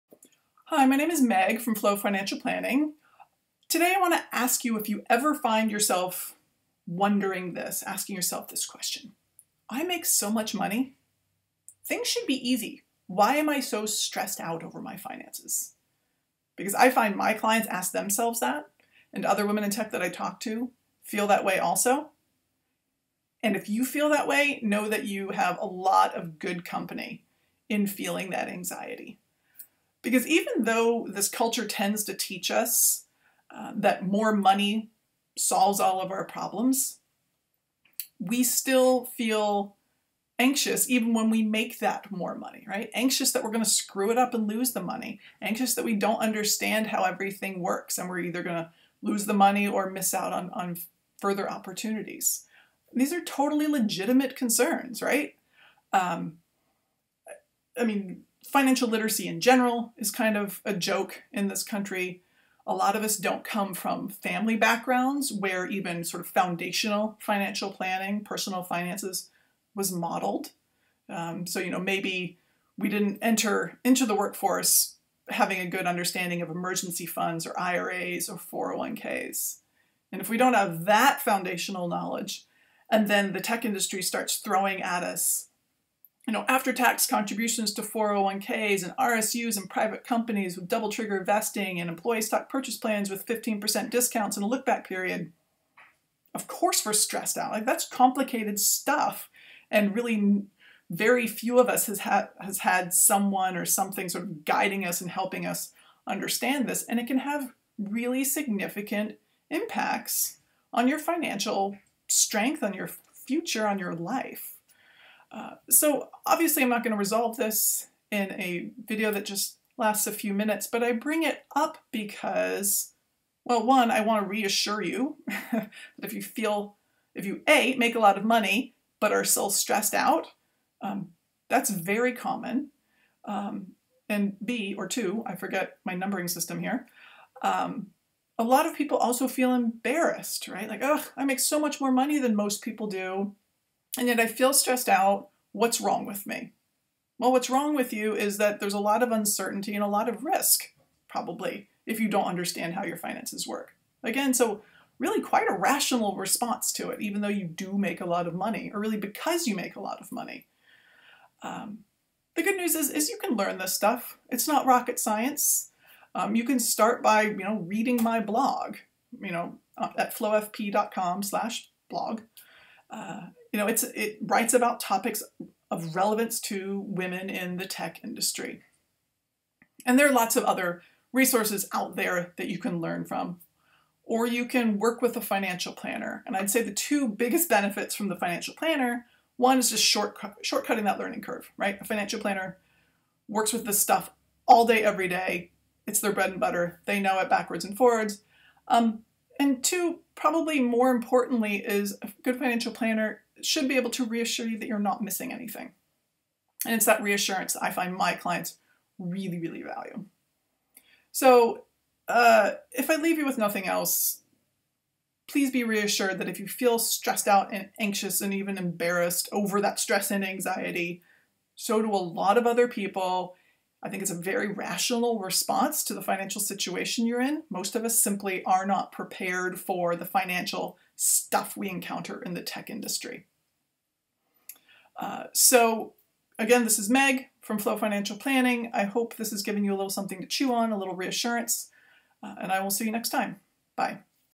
I Make So Much Money. This Should Be Easy. Why Am I So Stressed Out?
It's easy to think that if you make a lot of money, your financial life should be easy. But if you're anxious, you have every reason to be! Finances in tech are straight up complicated. And the risks of loss or lost opportunity are real.